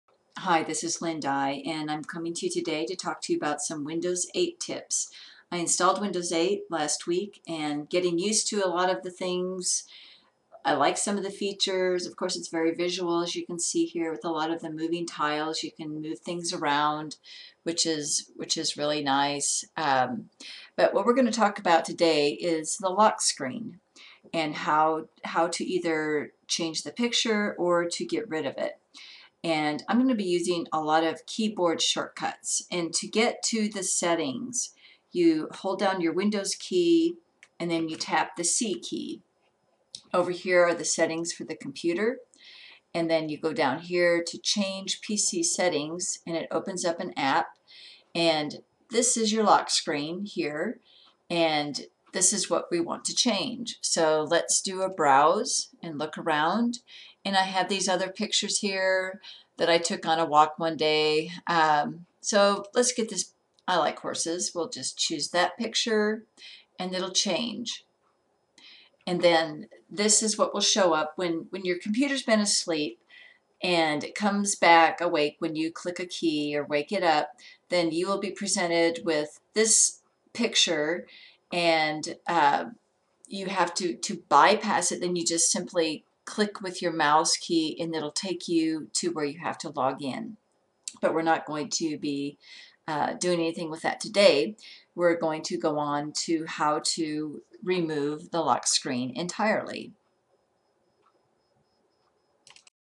Windows 8-Change Lock Screen Picture OR Disable Lock Screen
In this video, I'll show you how to either change your lock screen (get rid of the Seattle default pic), OR just get rid of seeing the lock screen entirely.